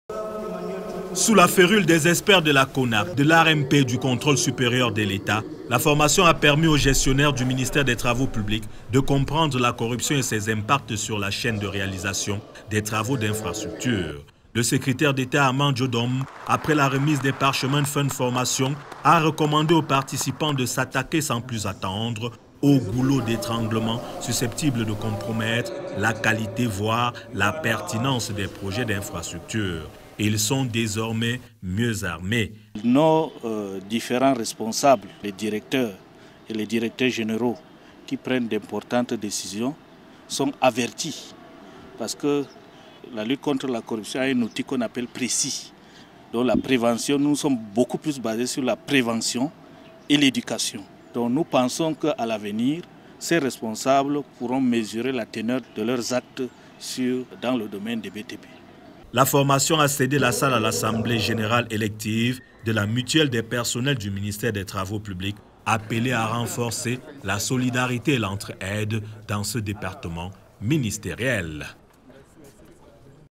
MINTP: ANTI CORRUPTION
Compte-rendu du séminaire portant sur le renforcement de capacités du personnel du MINTP dans la lutte contre la corruption.
séminaire tenu à l'ENSTP de Yaoundé